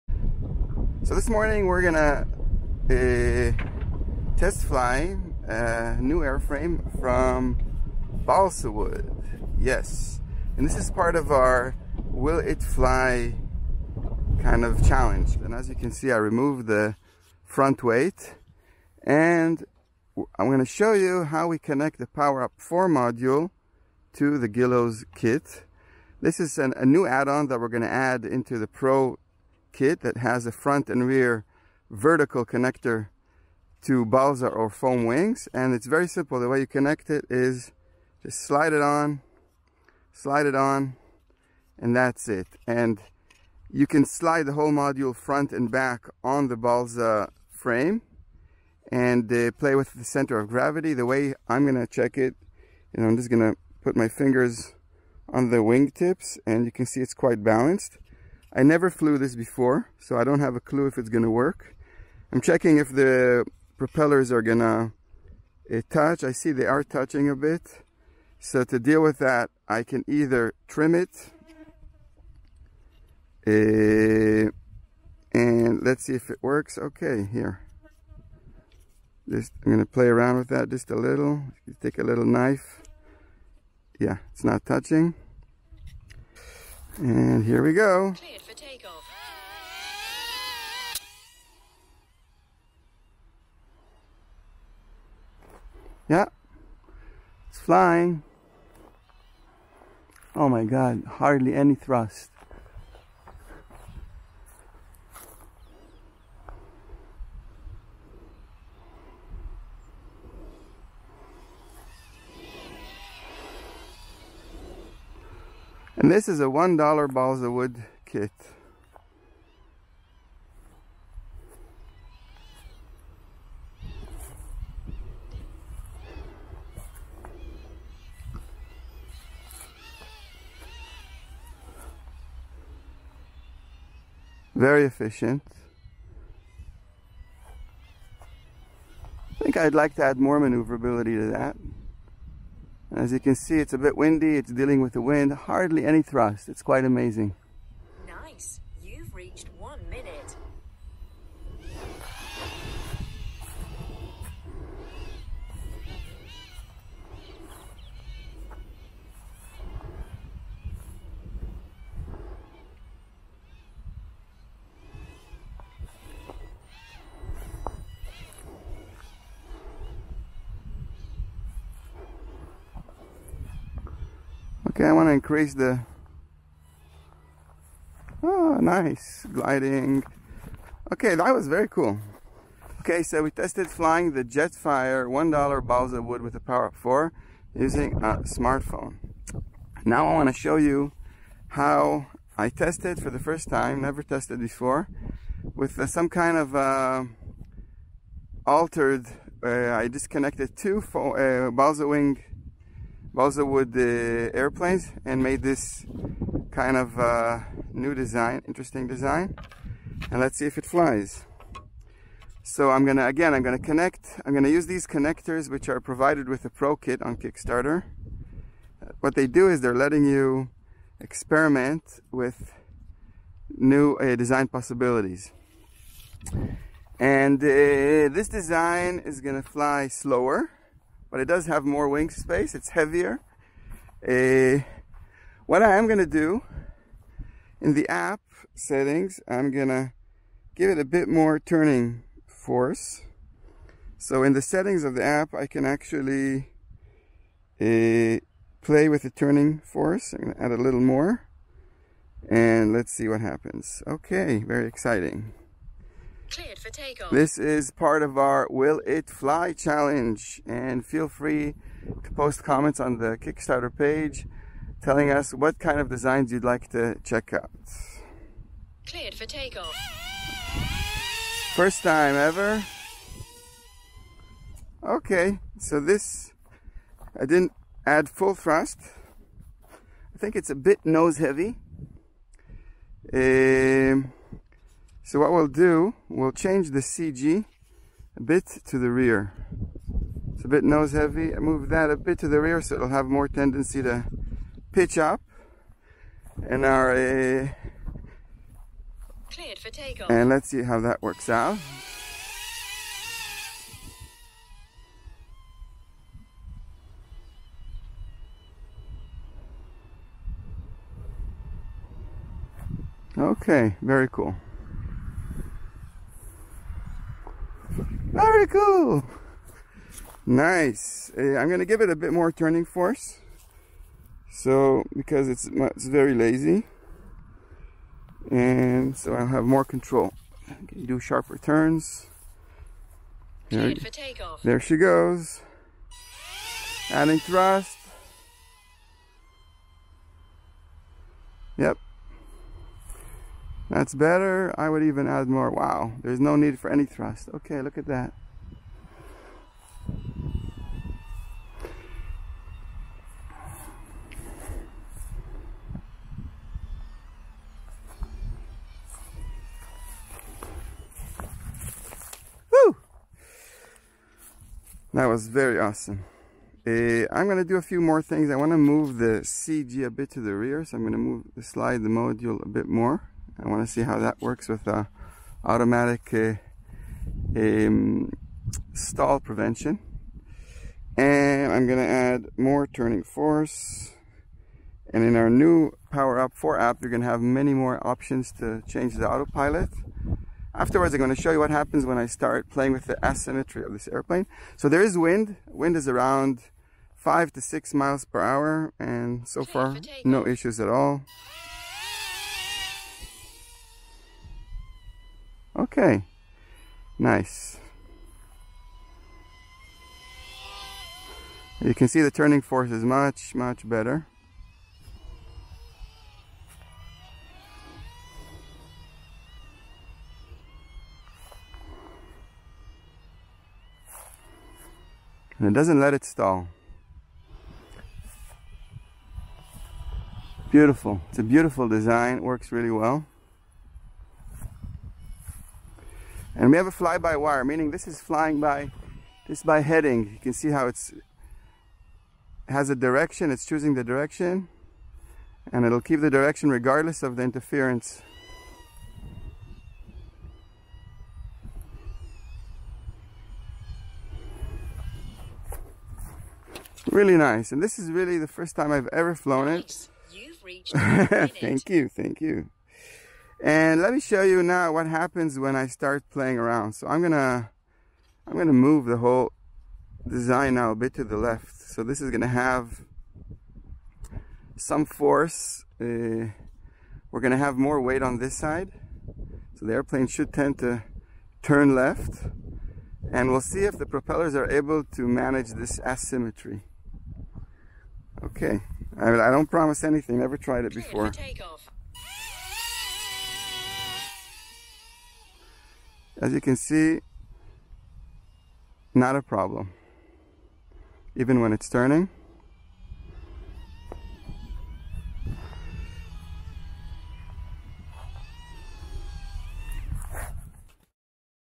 Will it fly challenge with POWERUP 4.0 using balsa-wood gliders
Discover what POWERUP 4.0 Smartphone and sensor controlled paper  airplane kit can drive and fly.
This time we are exploring the Guillows $1 childhood balsa planes which thanks to our 4.0 conversion kit can now be controlled and do aerobatics.